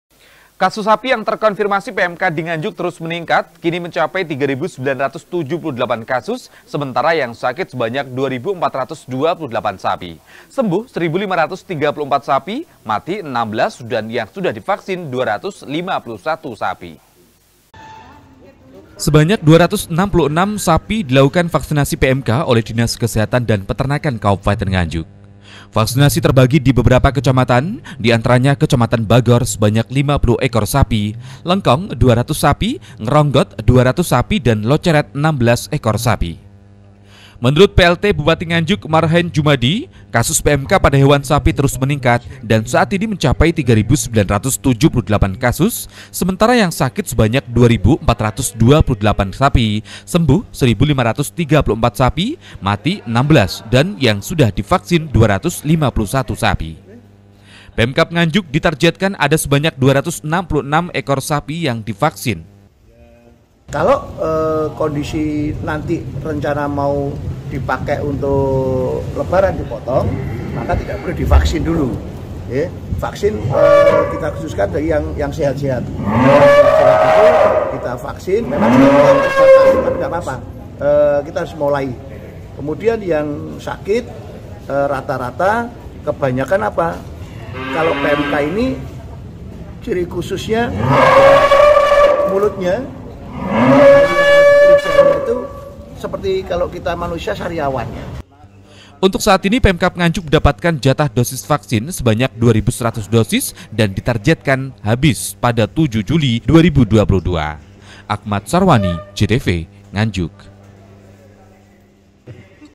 NGANJUK - PEMKAB Dijatah Stok Vaksin 2100 Dosis
Kasus sapi yang terkonfirmasi pmk dinganjuk terus meningkat kini mencapai 3978 kasus sementara yang sakit sebanyak 2428 sapi. Sembuh 1534 sapi. Mati 16 dan  yang sudah di vaksin 251 sapi. Dan hari ini pemkab nganjuk menargetkan ada sebanyak 266 ekor sapi yang di vaksin.